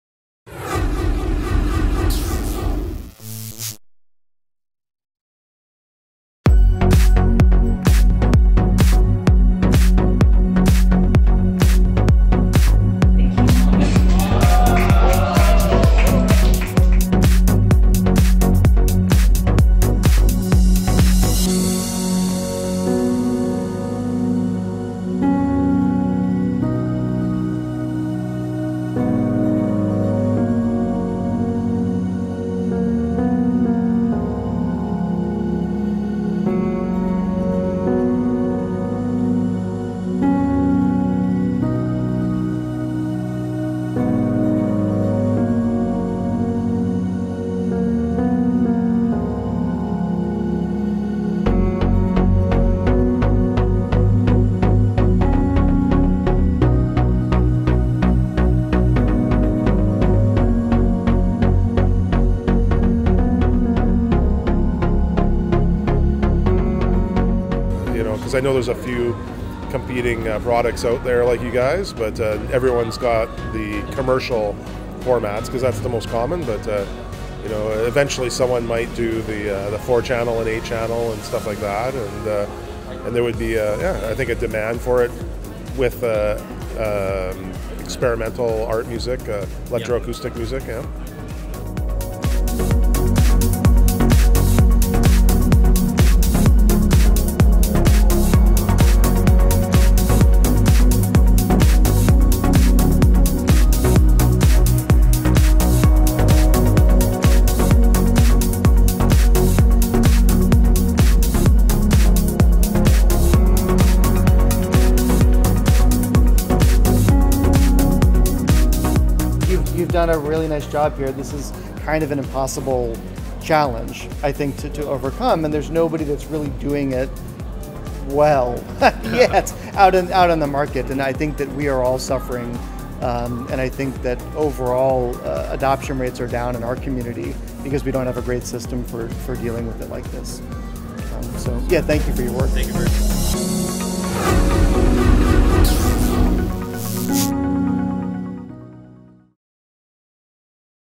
[ICMC 2018] 기록영상
소닉티어는 2018년 8월 5일부터 10일까지 대구에서 개최된 ICMC(국제컴퓨터음악컨퍼런스) 2018에 참가하였습니다.

현장의 생생한 분위기를 느껴보세요.